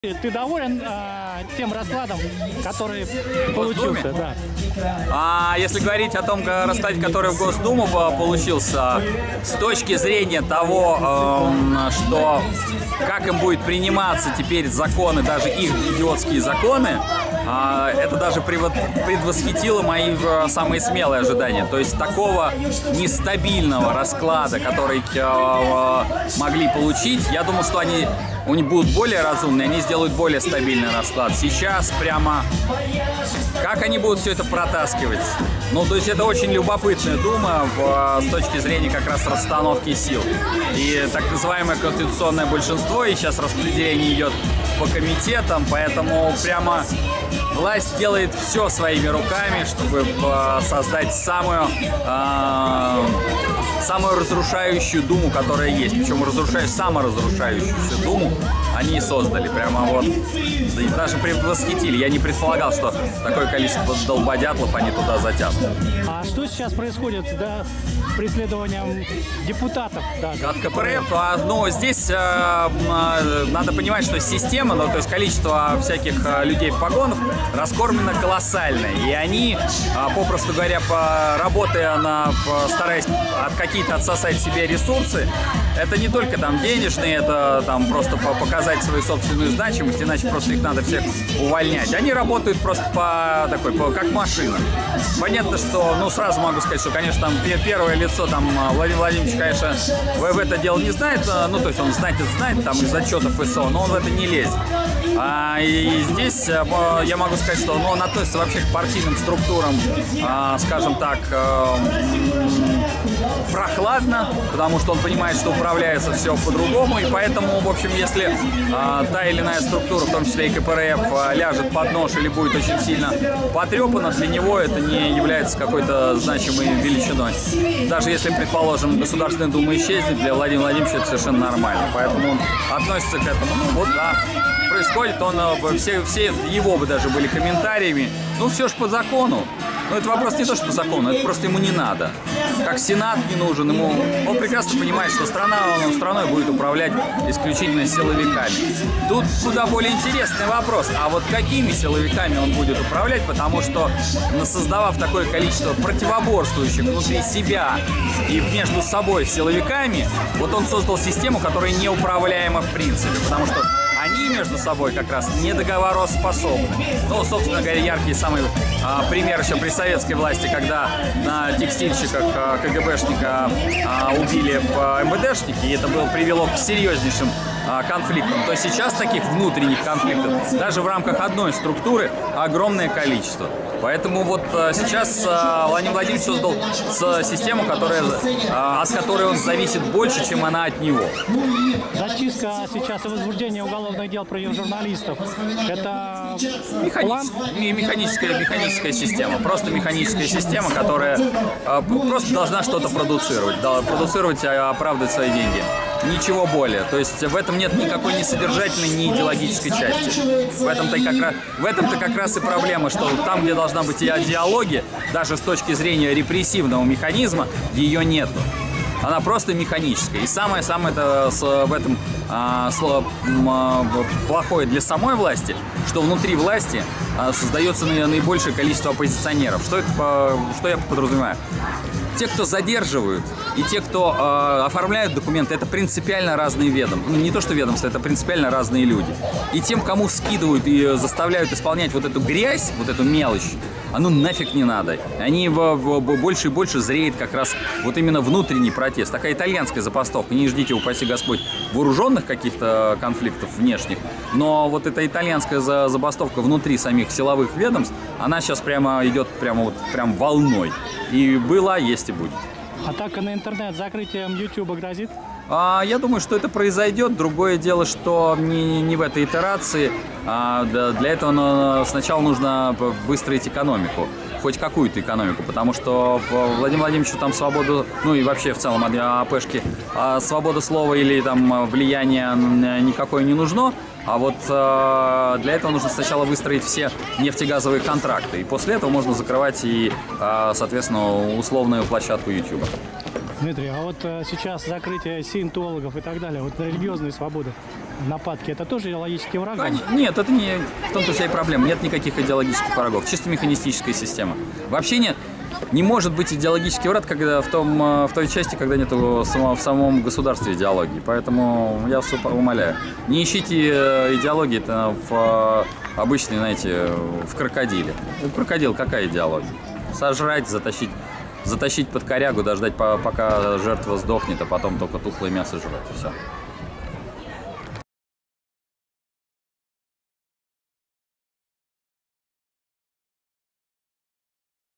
Как власть разрушает сама себя изнутри | Дмитрий Потапенко
Известный общественный деятель Дмитрий Потапенко прокомментировал прошедшие выборы и ответил на вопросы нашего канала ТВ Совхоз. Эксперт назвал состав вновь избранной Государственной Думы саморазрушающимся. Он подчеркнул, что преследование журналистов и представителей не православной конфессии не имеет идеологической основы и порождает оппозиционные взгляды внутри самой власти.